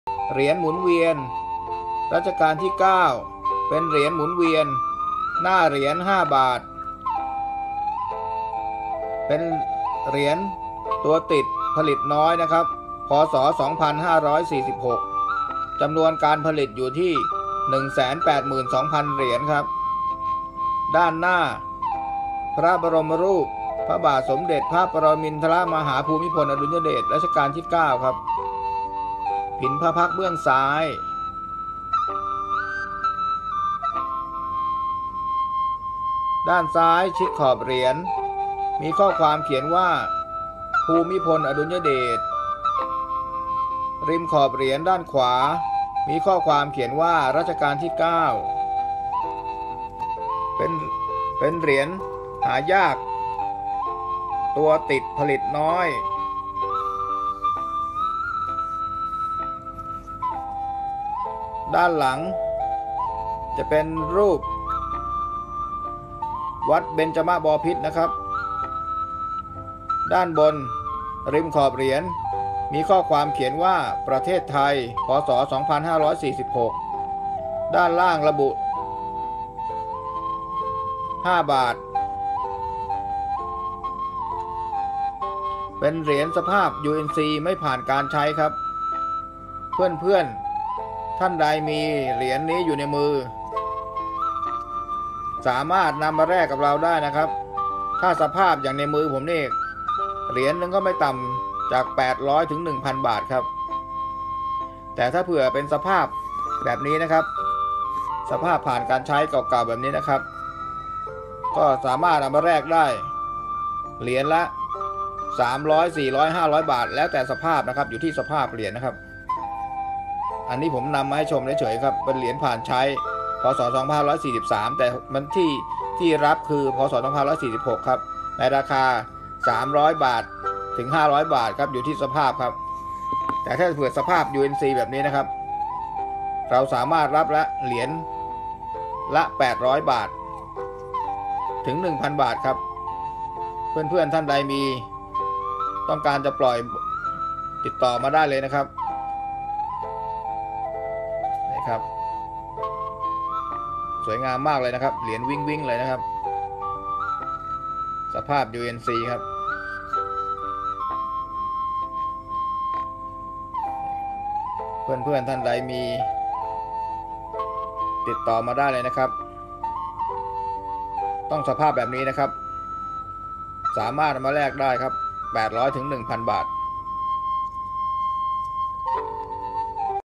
เหรียญ ๕ บาท แบบใช้หมุนเวียน รัชกาลที่๙ พ.ศ.๒๕๔๖ สภาพเหรียญสวยUNC นำมาแรกได้ เหรียญละ ๑,๐๐๐ บาท
ความรู้เรื่องของสะสมต่างๆ เหรียญและธนบัตร ราคาซื้อขายในปัจจุบัน แนวทางในการเก็บสะสม วิธีเก็บรักษา